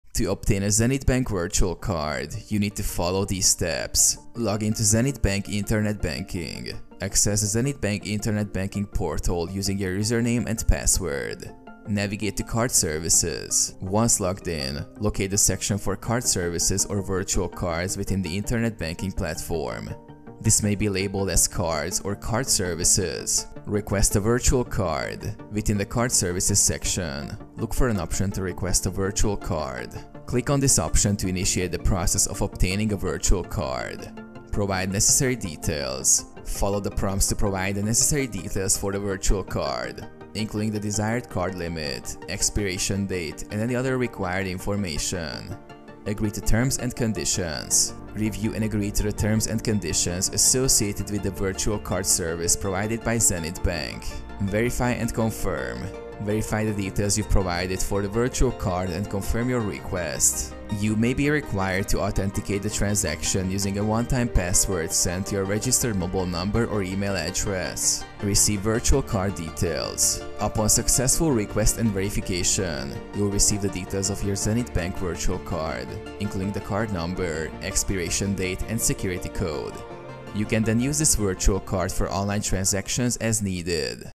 How To Get Zenith Bank Virtual Card? (2024)
In this video I will explain How You Can Get Zenith Bank Virtual Card In 2024.